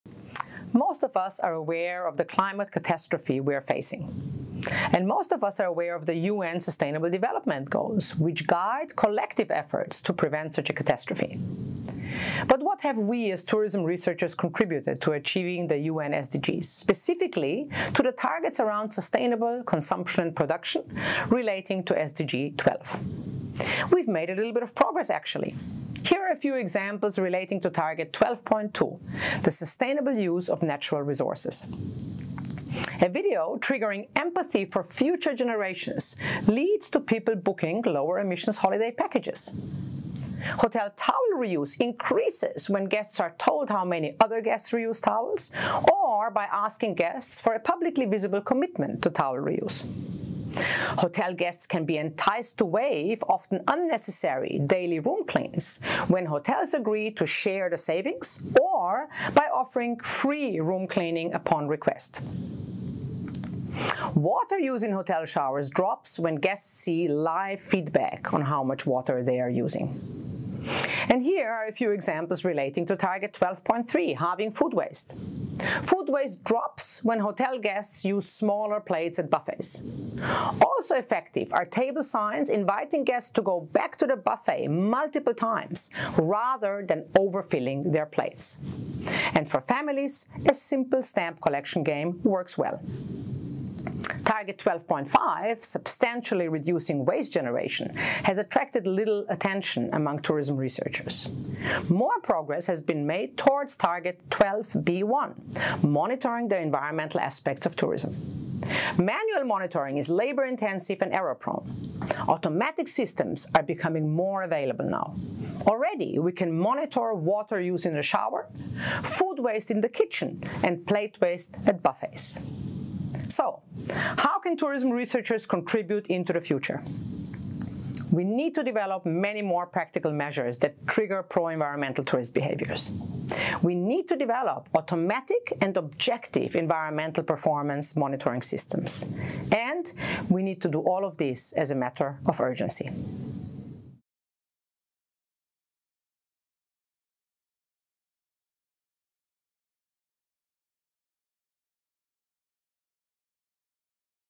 Tourist behaviour change for sustainable consumption SDG Goal12
Purpose
The purpose of this perspective paper is to draw attention to how the tourism industry can take action to contribute to United Nations Sustainable Development Goal 12 by nudging tourists to behave in more environmentally sustainable ways. Many of the available practical approaches are easy and cheap to implement and therefore can be deployed easily by small and medium-sized tourism businesses.

Design/methodology/approach
This perspective paper reviews past contributions to United Nations Sustainable Development Goal 12 and calls for more future research that develops and empirically test effective behavioural change interventions.

Findings
Future research must focus on two directions: the development and empirical testing in the field of new behavioural change interventions that entice tourists to behave in more environmentally sustainable ways, and the development and large-scale deployment of affordable instrumentation to automatically and continuously measure the environmental performance of tourism businesses.

Originality/value
This perspective paper synthesizes literature and point to important new future research directions. To the best of the authors’ knowledge, this is the first synthesis of past tourism research that has developed and empirically tested tangible approaches the tourism industry can deploy to contribute to SGD 12.